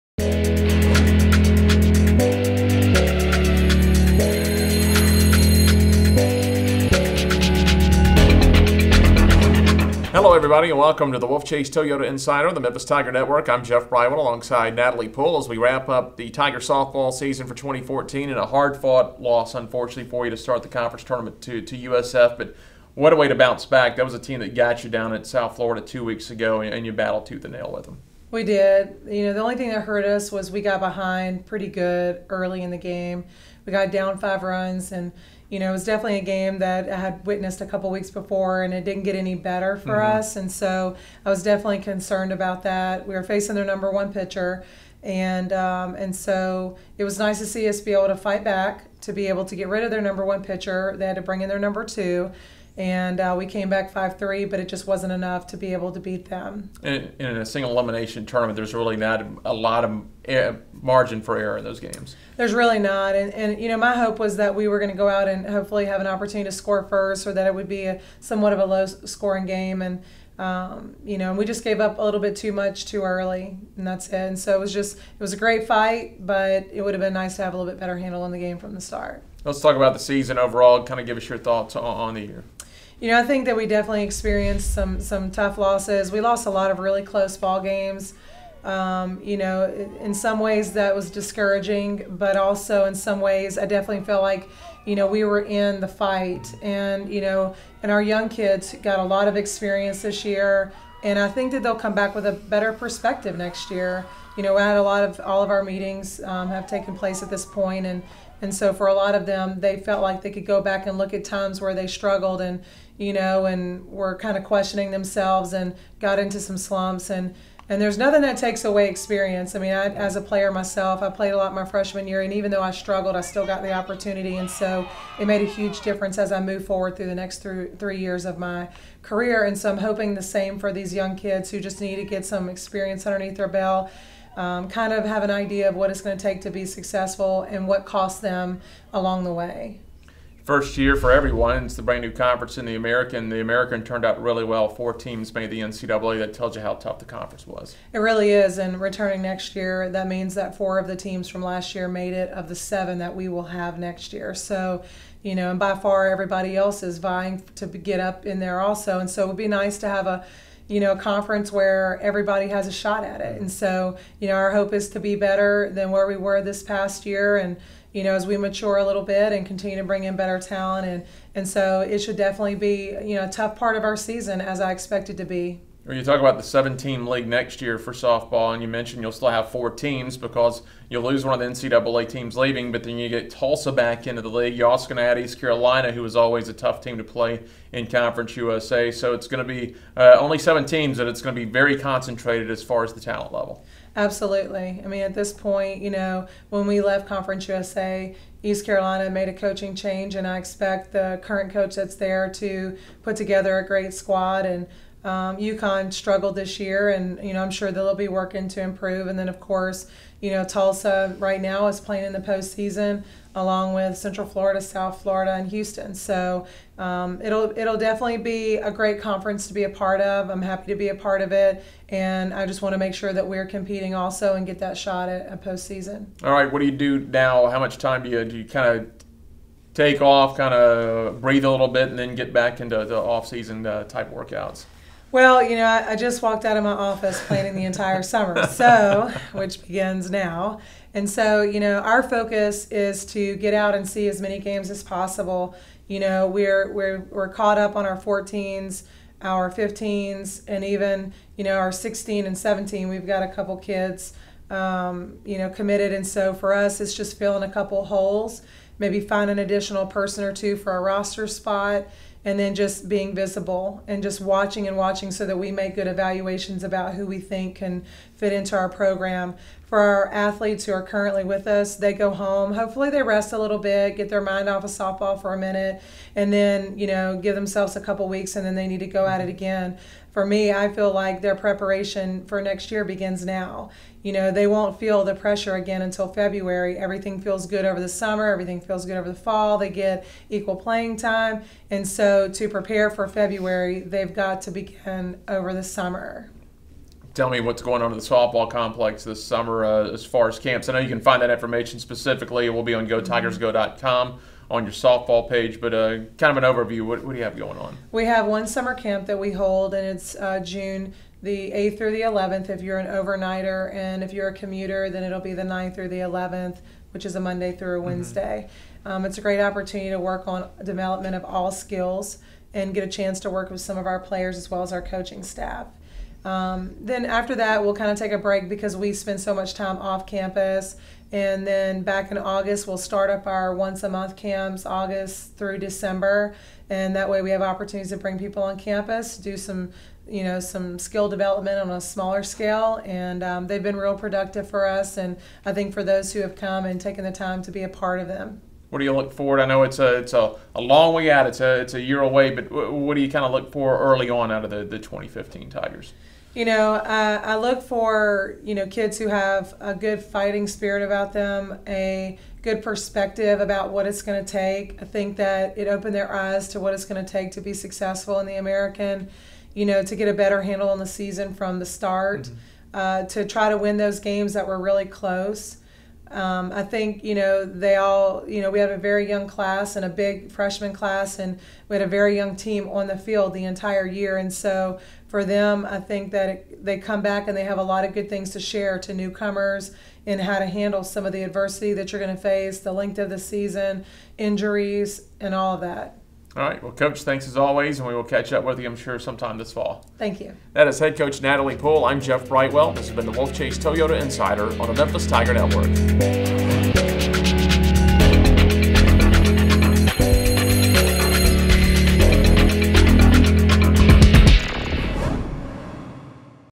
Wolfchase Toyota Insider: Memphis Softball
Head Softball Coach Natalie Poole joins Jeff Brightwell to discuss the Tigers final game against USF in the American Athletic Conference Tournament as well as looking ahead to summer planning and next season.